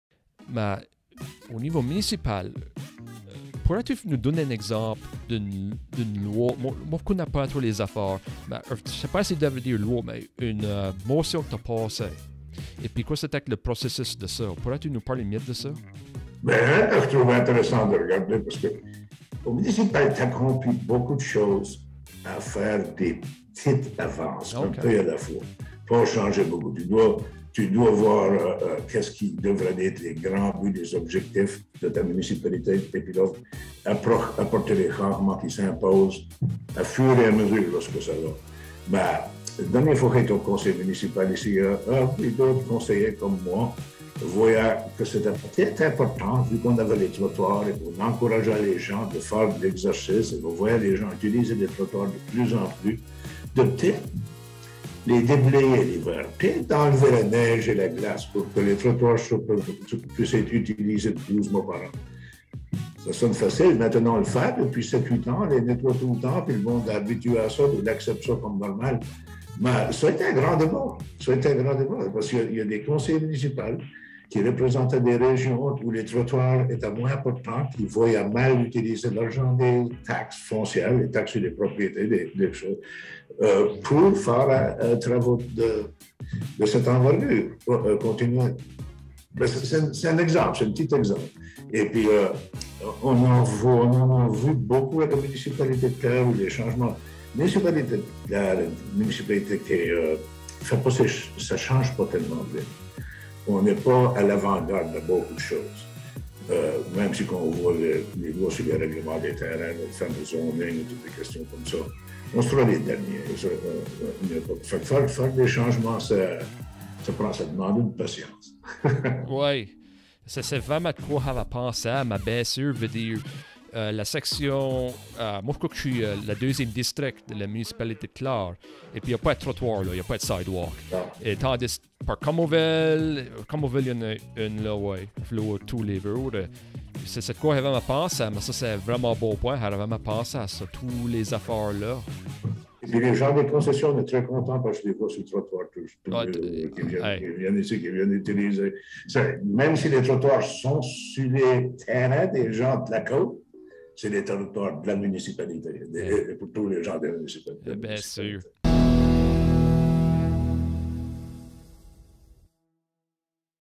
Robert Thibault - "Les politiques municipales et fédérales"
Robert Thibault nous donne une example des différences entre les politiques municipales et fédérales.